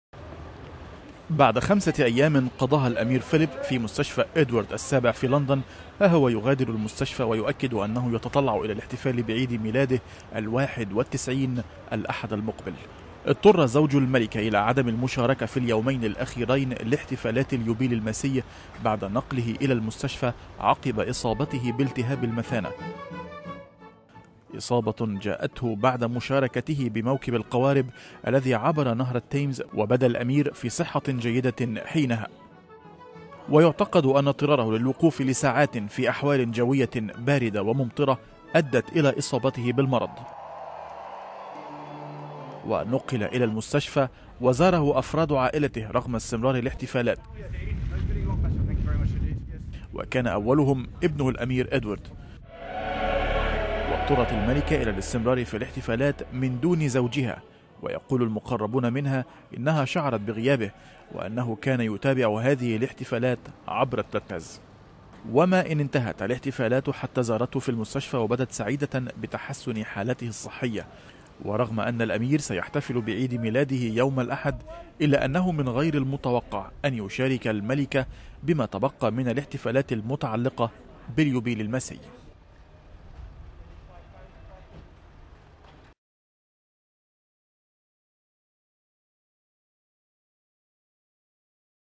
دوق ادنبره الامير فيليب يغادر المستشفى
غادر دوق ادنبره الامير فيليب زوج الملكة البريطانية اليزابيث الثانية مستشفى الملك ادوارد السابع في لندن بعد ان قضى خمسة ايام فيها.